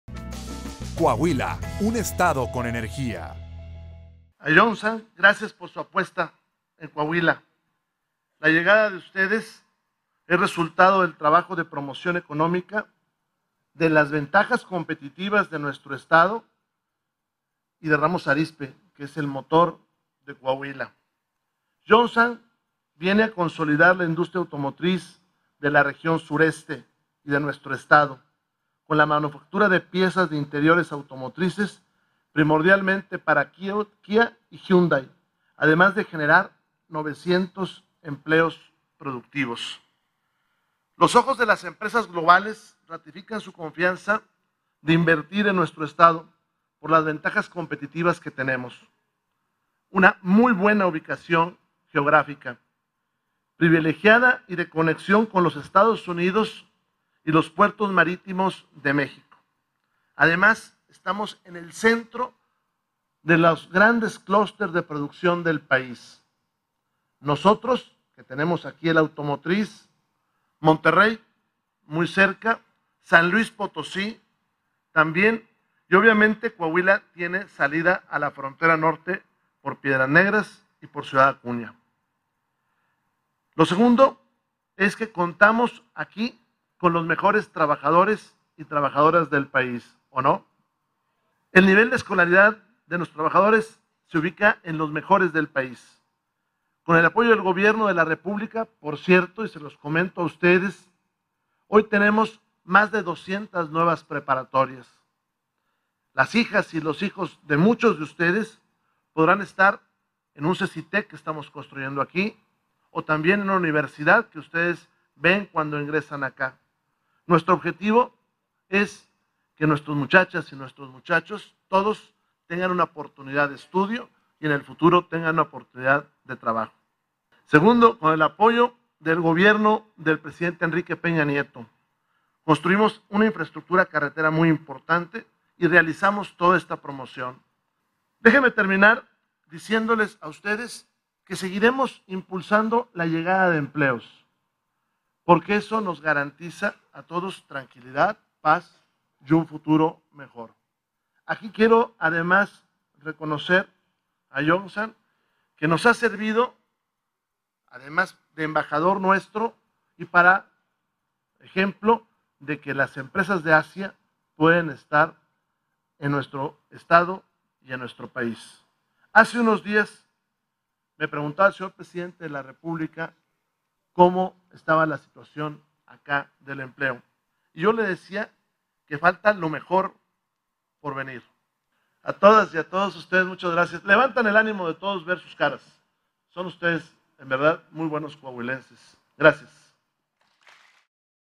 Inaugura Rubén Moreira la empresa Yongsan Automotive de México
El Gobernador del Estado Rubén Moreira Valdez, sostuvo que la llegada de la empresa coreana Yongsan Automotive de México a Ramos Arizpe es producto del trabajo y la consolidación de Coahuila como el mejor destino de inversiones.